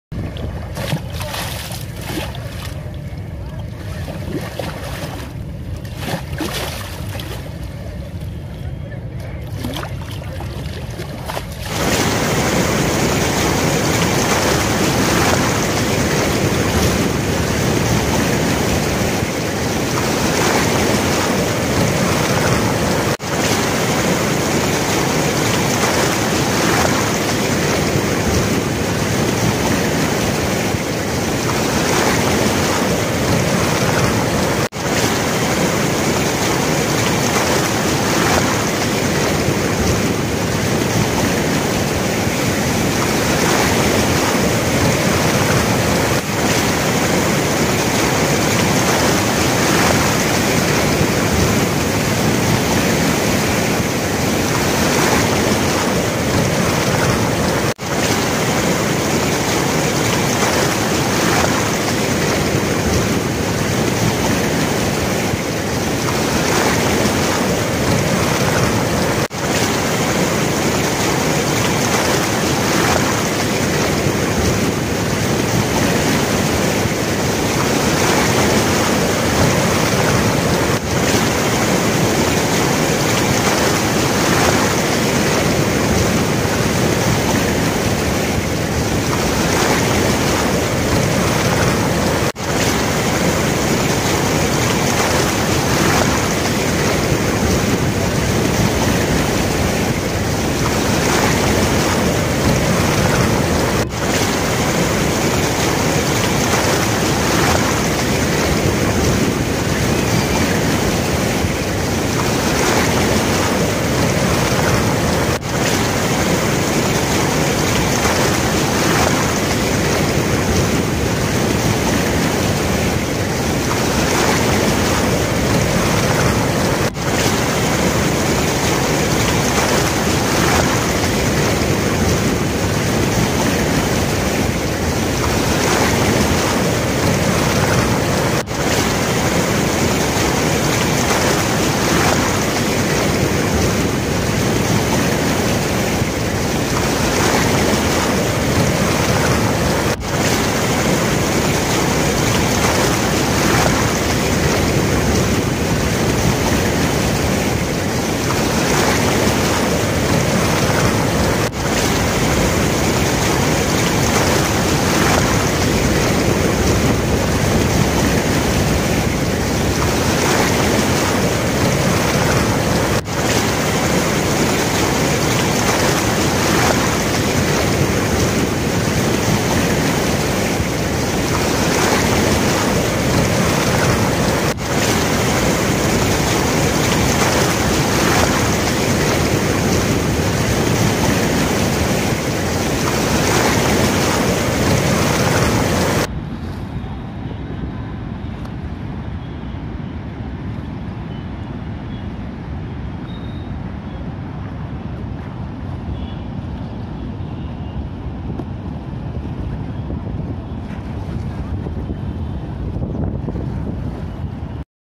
Krishna River Prakasam Barrage Flood Exclusive Video || 70 gates lifted || social media live
Exclusive Video Krishna River Prakasam Barrage Flood Today || 70 gates lifted || social media live
పరవళ్లు తొక్కుతూ ప్రవహిస్తున్న కృష్ణమ్మ || krishna river prakasam barrage || social media liv
కృష్ణమ్మ పరవళ్లు*

కృష్ణా బ్యారేజ్ వద్ద పరవళ్లు తొక్కుతూ ప్రవహిస్తున్న కృష్ణమ్మ...

విజయవాడ ప్రకాశం బ్యారేజ్ వద్ద అంతకంతకు పెరుగుతున్న వరద ఉధృతి...

ఎగువున కురుస్తున్న వర్షాలకు బ్యారేజ్ కు పెద్ద ఎత్తున వస్తున్న వరద నీరు...

ప్రకాశం బ్యారేజ్ 70 గేట్లు ఎత్తి సముద్రంలోకి వరదనీరు వదులుతున్న అధికారులు..